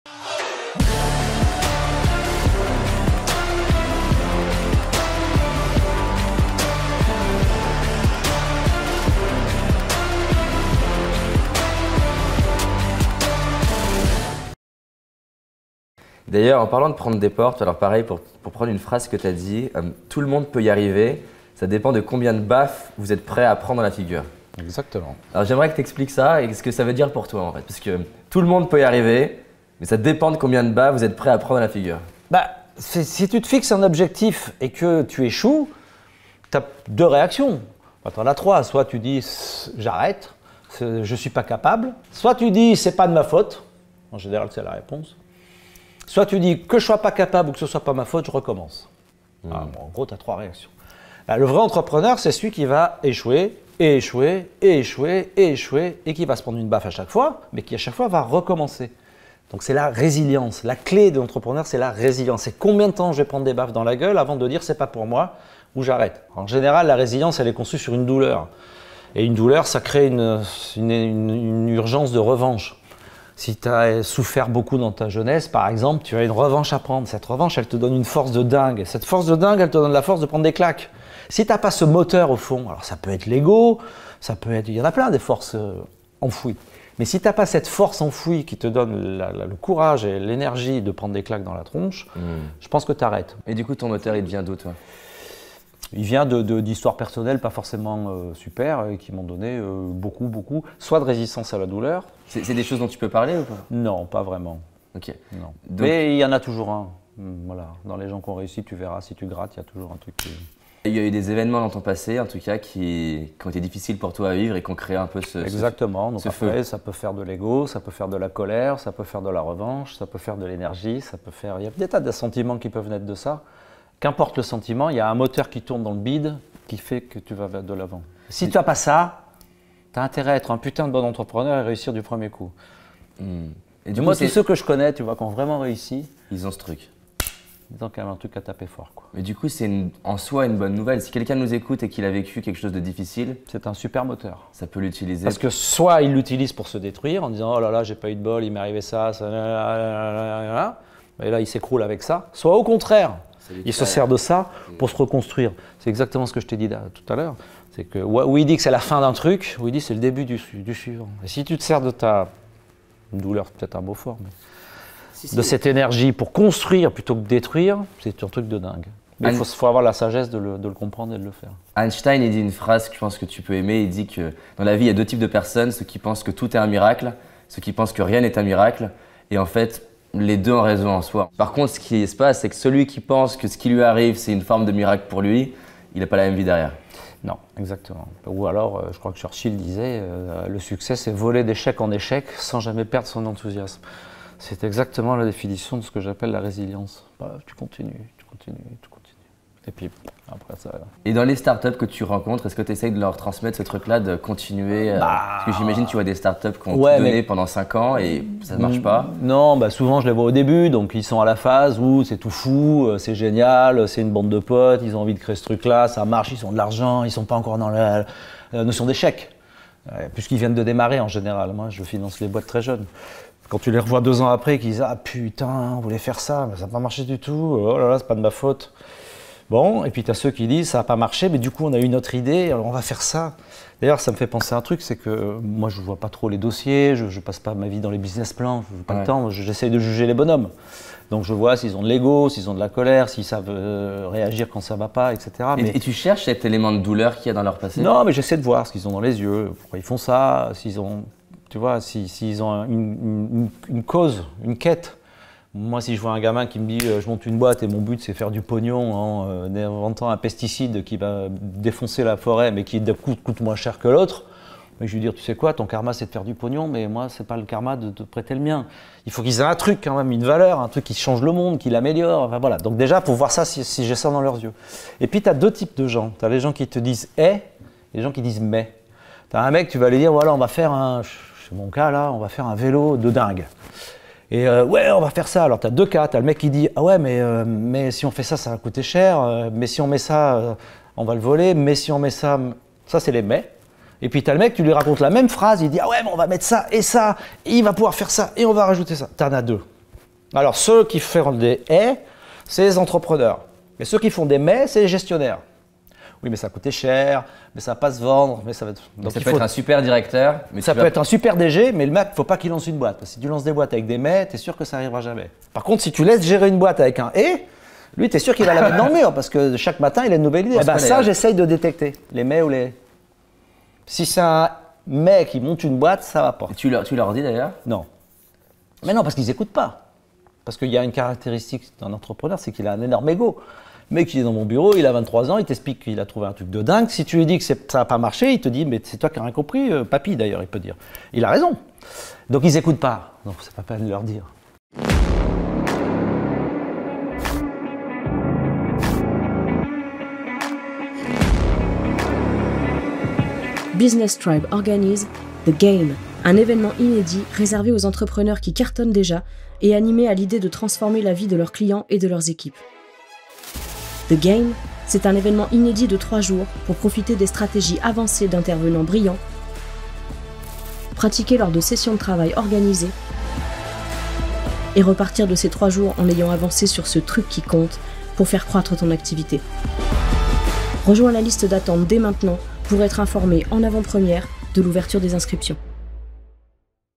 #27 Marc Simoncini : La réussite dépend du nombre d'échecs que tu es prêt à endurer
Comment faire preuve de résilience ?

Marc Simoncini est le fondateur de Meetic puis de Sensee, investisseur et producteur dans le cinéma. Cette réussite, il la doit à son inlassable persévérance, allant d’échec en échec sans jamais abandonner.

Dans cet épisode Marc nous partage comment il fait face aux échecs et se relève sans cesse.

Clique ici si tu veux en savoir plus sur le prochain événement business